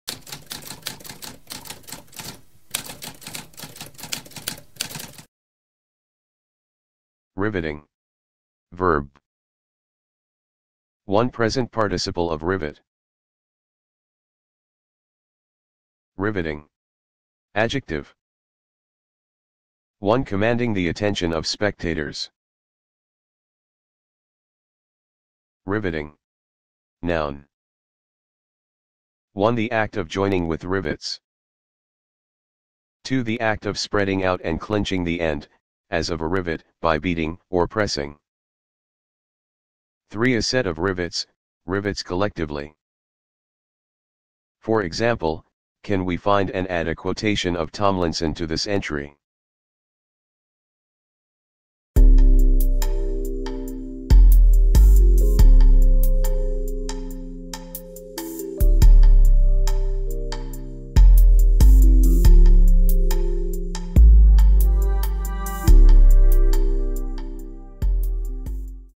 What does riveting mean?
A spoken definition of riveting.

Intro Sound:
Typewriter - Tamskp